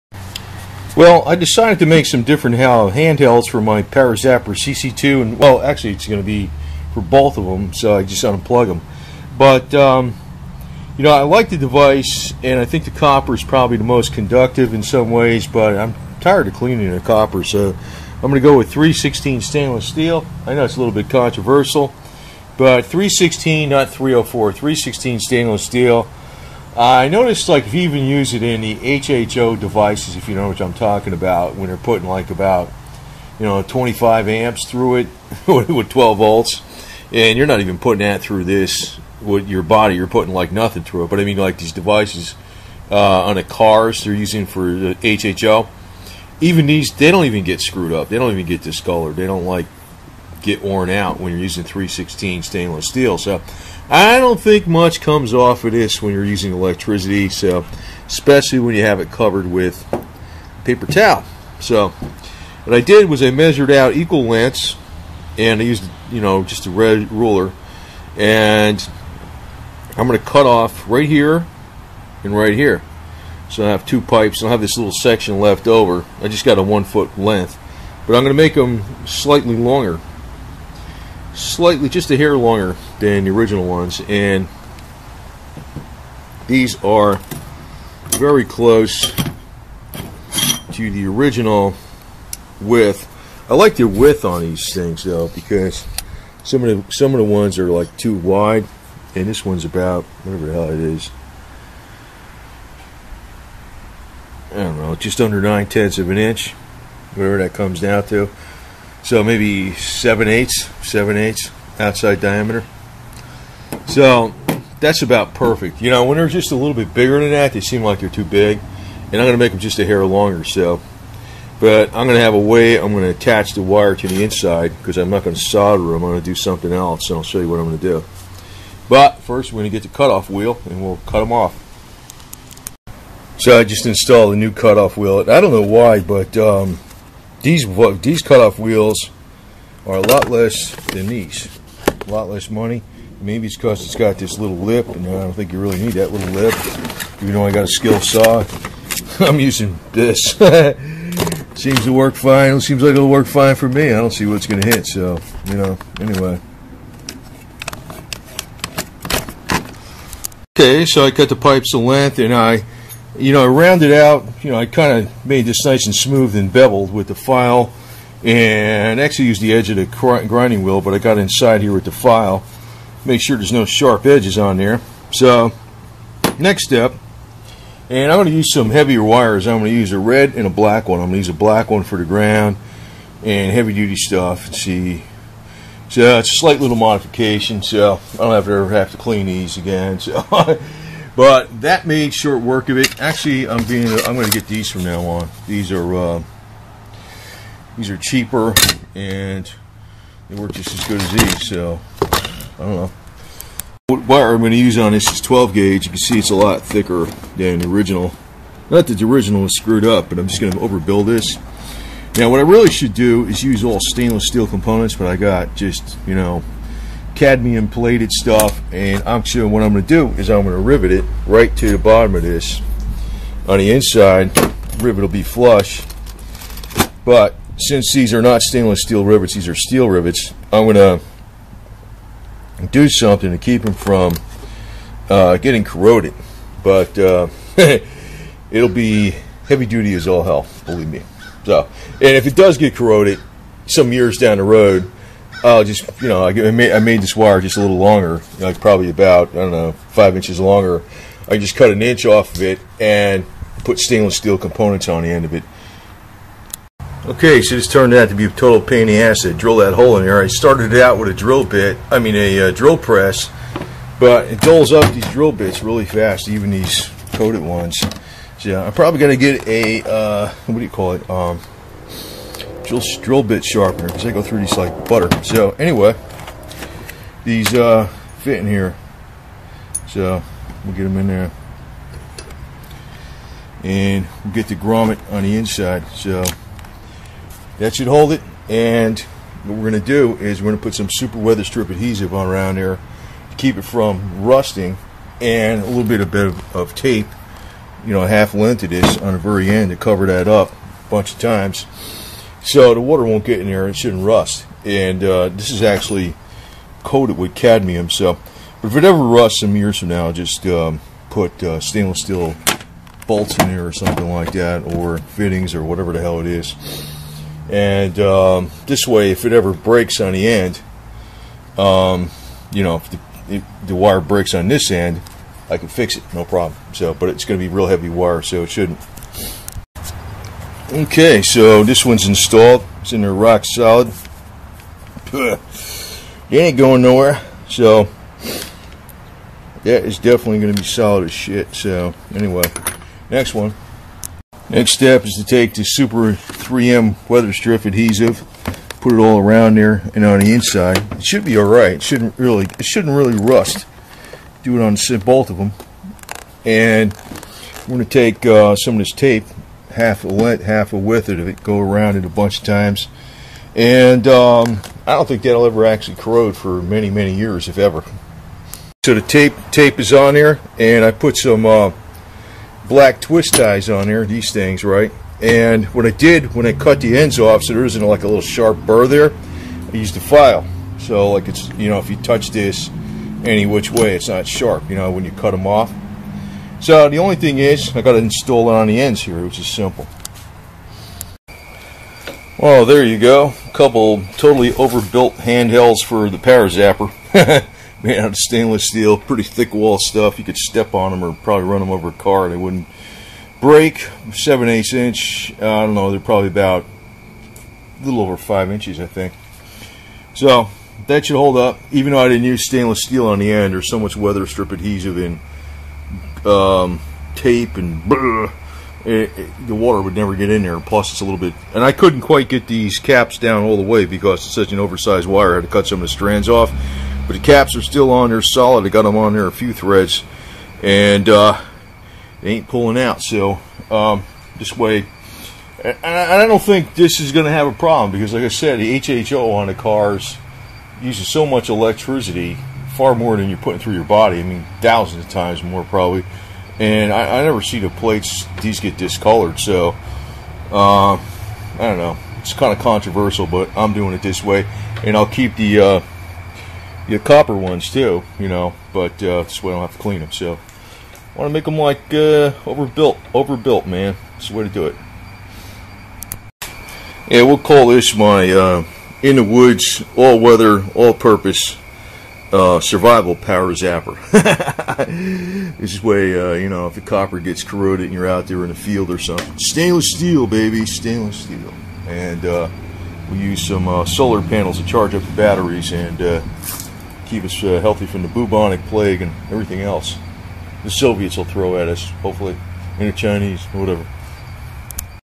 Make STAINLESS Steel Handhelds for the ParaZapper
It is somewhat controversial but some say to only use copper of put silver (or gold) for the the handheld on the zapper. but others says 316 stainless steel is good too.  I decided to make some hand-held for the Parazapper since they will never tarnish or require cleaning.  I used 316 stainless steel 7/8 inch thick wall tube.  I used 1/4 structural rivets to attach the wires to the stainless steel tubed so it is a extremely strong connection.  I waterproofed the connection with weatherstrip adhesive ans used 12 gauge wire.  It should last many years and will never tarnish, so it never needs to be cleaned.